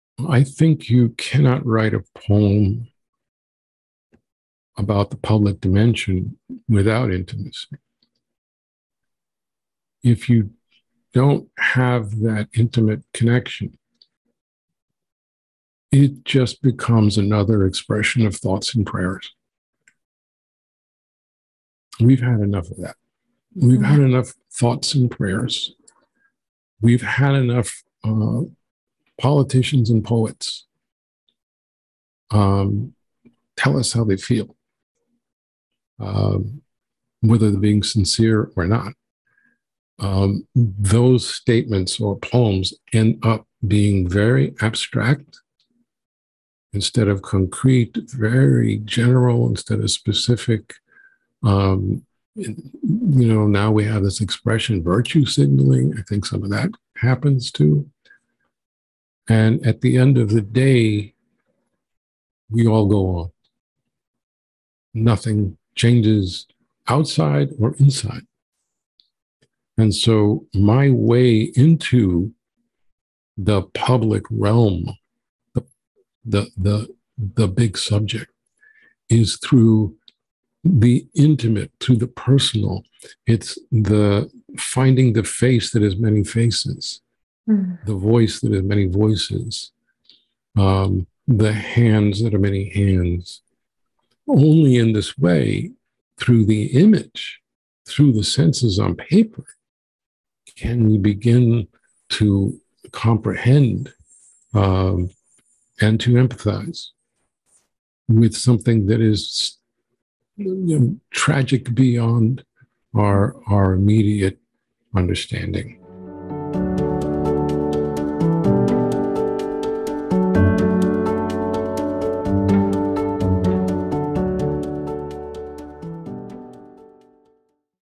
Intimacy In the Poetry of Tragedy | Martín Espada | Collective Trauma Summit
A FREE 9-day online gathering with 50+ talks to share ideas and inspire action for healing individual, ancestral, and collective trauma.
"Times of hate call for poems of love." - Martín Espada

Martín Espada has published more than twenty books as a poet, editor, essayist, and translator. His latest book of poems is called Floaters, winner of the 2021 National Book Award and a finalist for the Los Angeles Times Book Prize. A former tenant lawyer in Greater Boston, Espada is a professor of English at the University of Massachusetts-Amherst. His talk, "The Gift of Finding Dignity in the Face of Pain" is part of the Collective Trauma Summit.